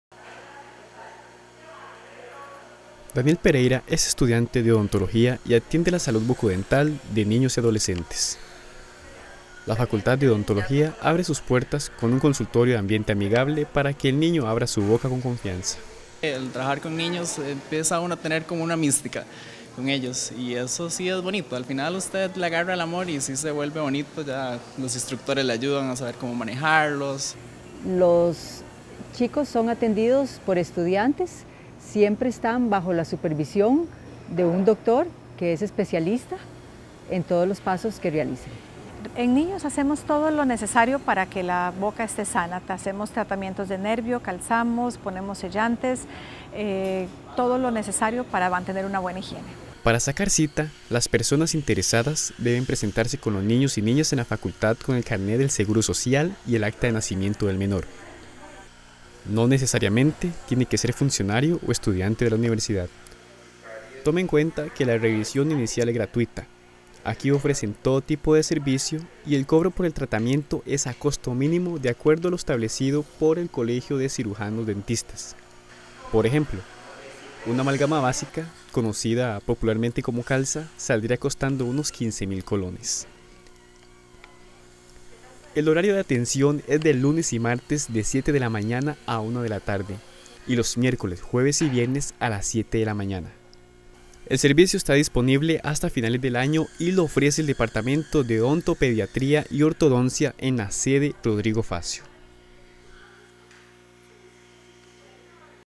Servicios de Odontología para los más pequeños
La proyección social de la UCR también saca sonrisas felices a la niñez y la adolescencia.
La Facultad de Odontología ofrece tratamiento bucodental a precios mínimos para todas las personas menores de edad que vengan acompañadas de un adulto.